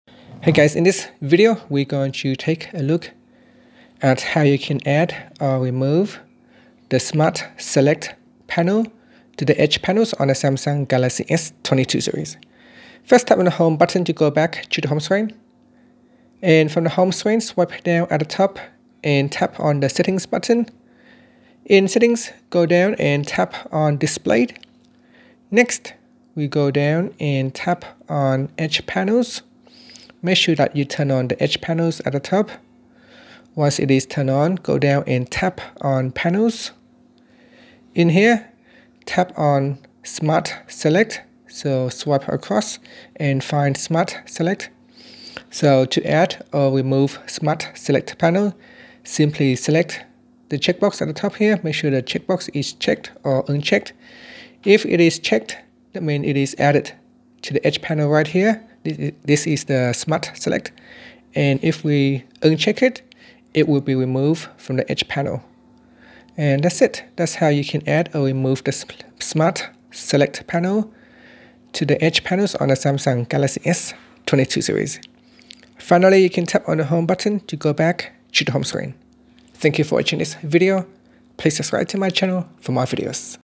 Samsung Galaxy S22/S22+/Ultra: How to Add/Remove SMART SELECT Panel to the Edge Panels
Learn how you can add or remove SMART SELECT panel to the Edge Panels on the Galaxy S22/S22+/S22 Ultra.